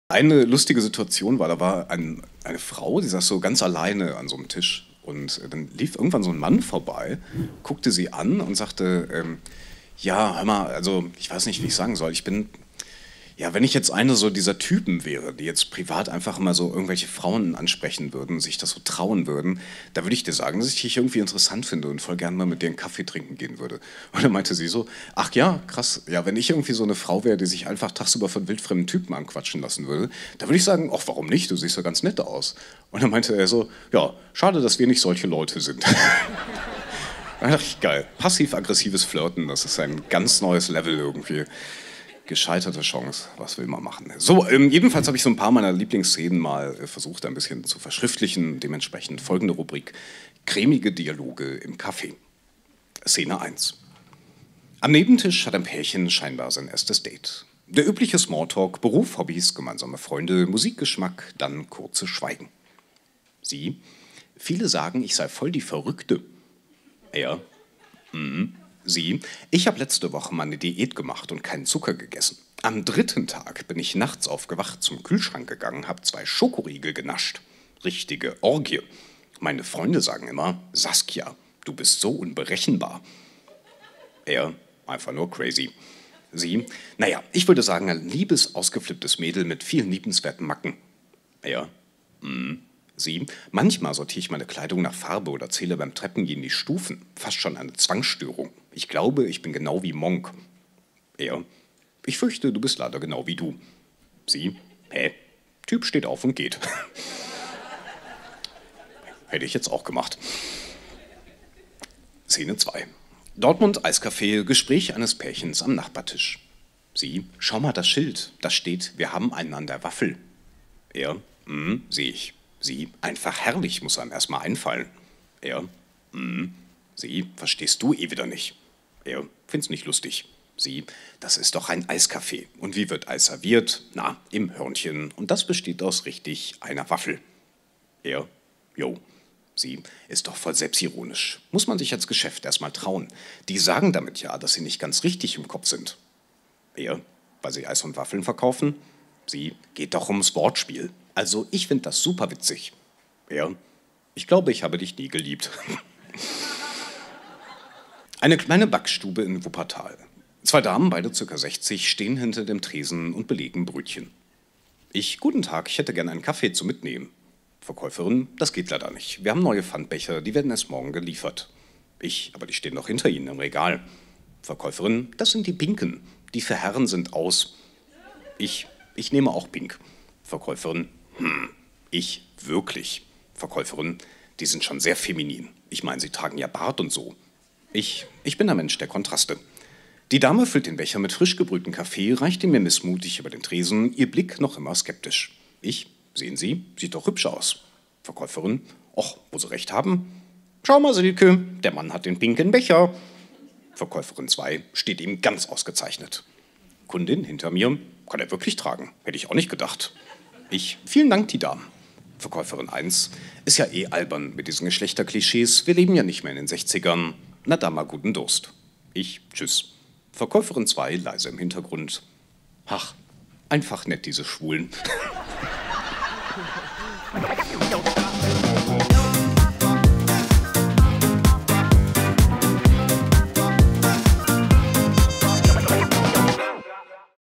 Patrick Salmen / Passiv agressives Flirten | Kleine Affäre
Passiv-agressives Flirten

Patrick Salmen ließt bei strömenden Regen bei der kleinen Affäre außer Haus. Ja, so ist er der Corona Open Air Sommer 2020